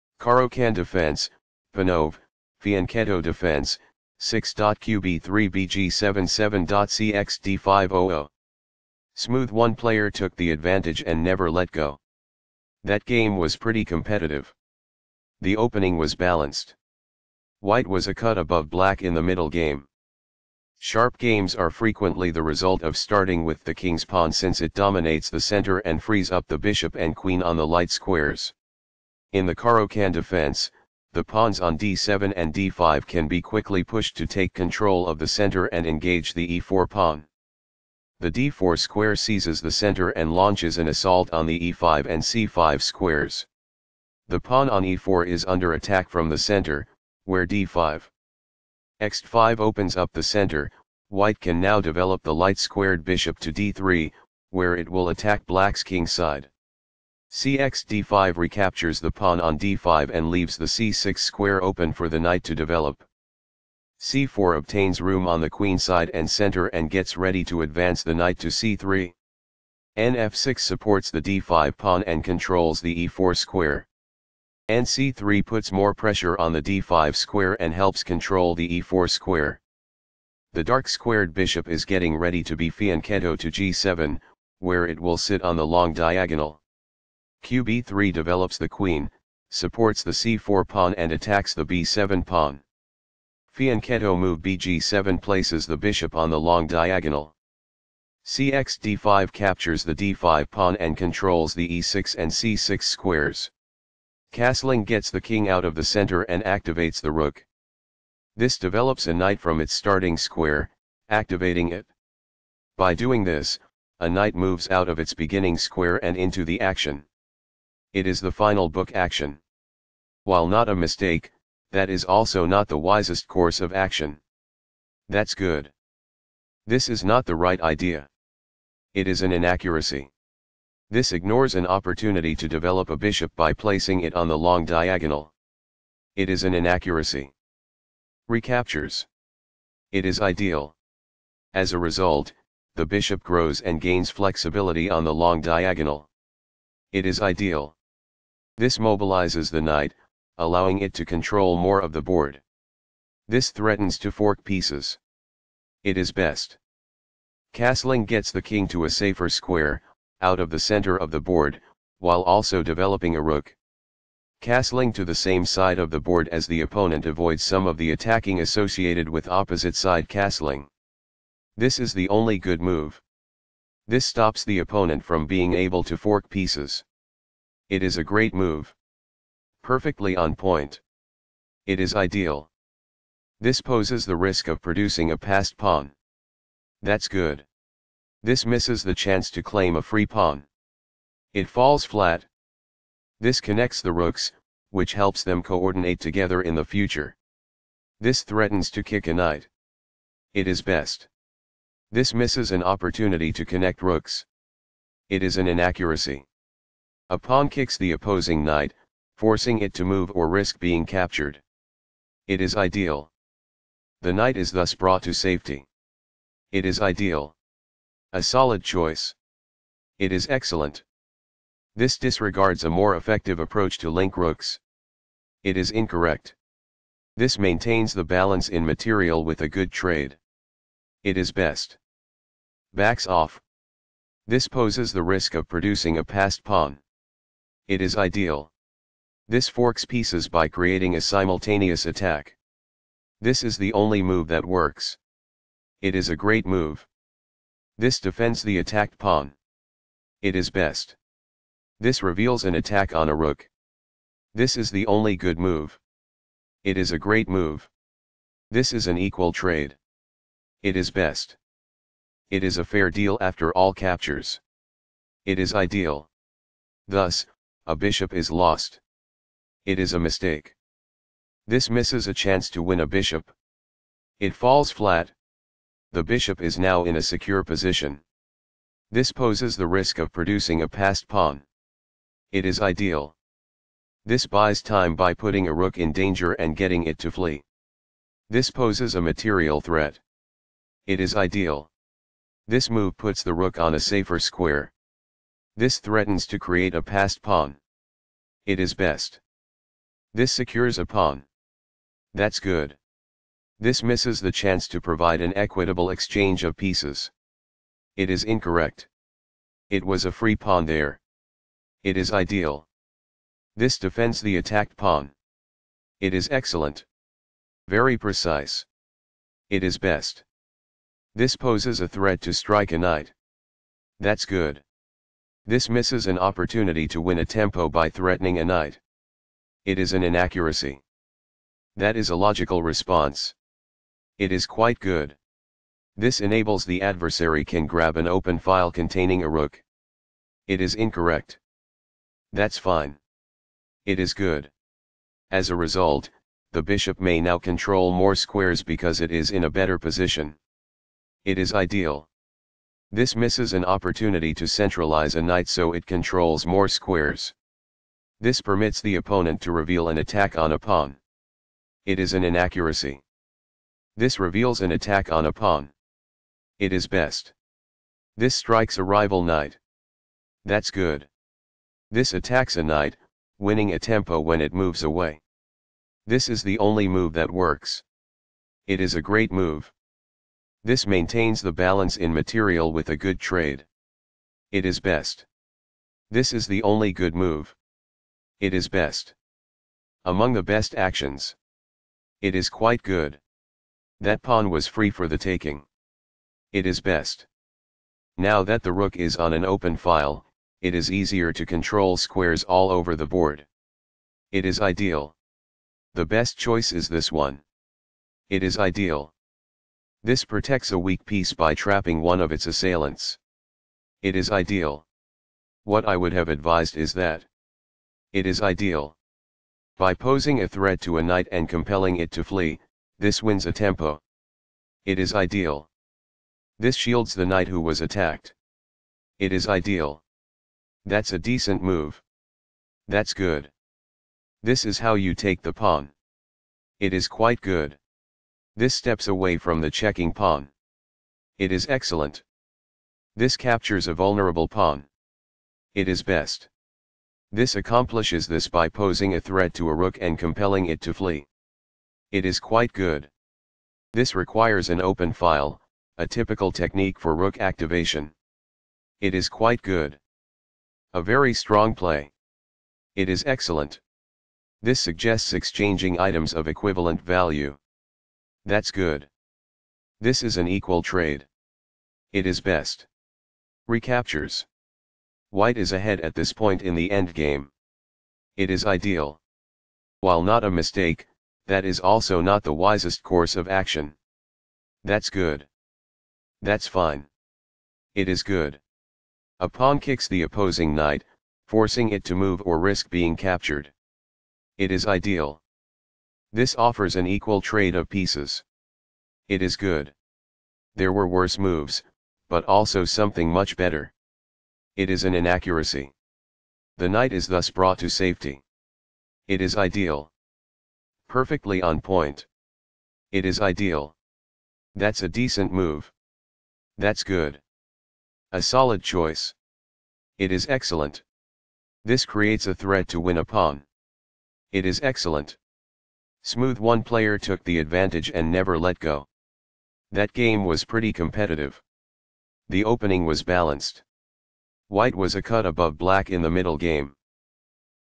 White Tal Mikhail (LAT), Black Botvinnik Mikhail M (RUS),Caro-Kann Defense: Panov, Fianchetto Defen
Caro-Kann Defense: Panov, Fianchetto Defense, 6.Qb3 Bg7 7.cxd5 O-O, Event Ch URS (team), Site Moscow (Russia), Date 1966, White Tal Mikhail (LAT), Black Botvinnik Mikhail M (RUS), Result 1-0, ECO B14, WhiteElo 0, BlackElo 2630, Remark